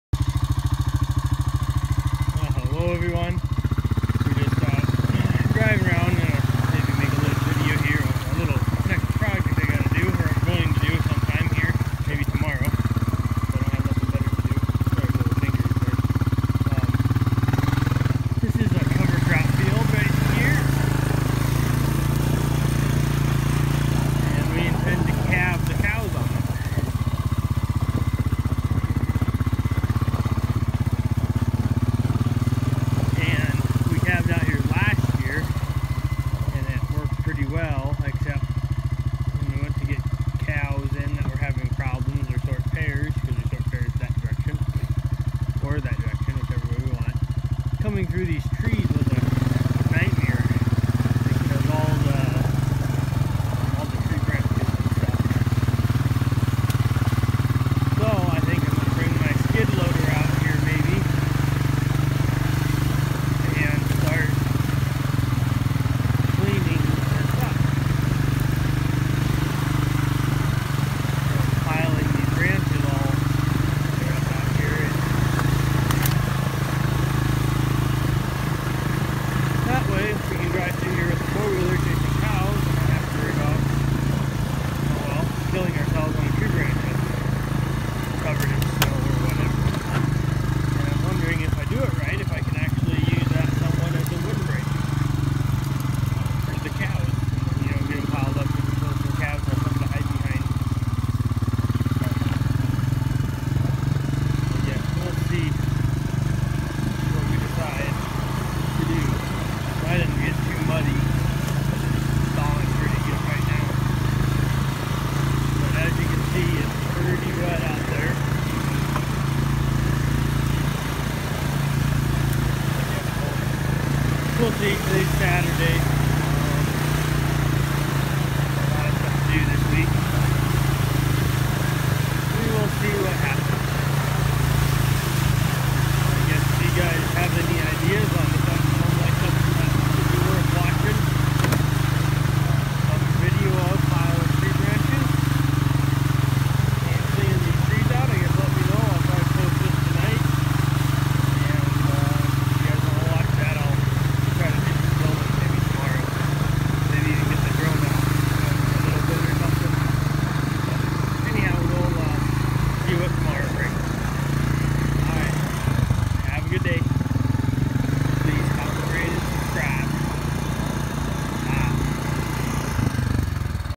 Next project Tree grove cleaning
Seeing if anyone would be interested in watching to do some tree grove cleaning with the skid steer